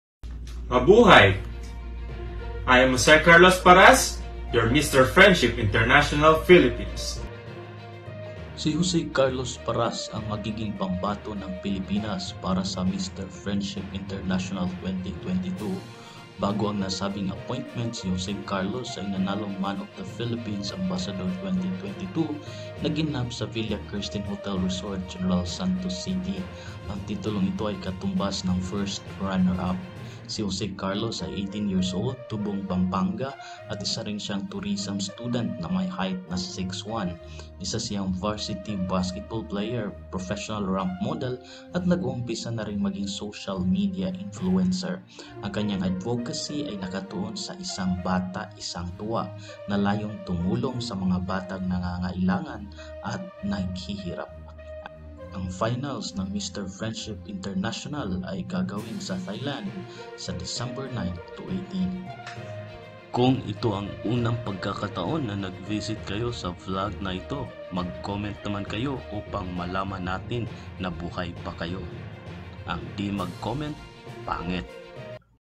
MISTER PHILIPPINES FOR MISTER FRIENDSHIP INTERNATIONAL | JOSE CARLOS PARAS
Jose Carlos Paras is the Philippine representative for the Mister Friendship International 2022.

MISS UNIVERSE SUPERFAN is a youtube vlog dedicated to beauty pageants around the world, especially Miss Universe.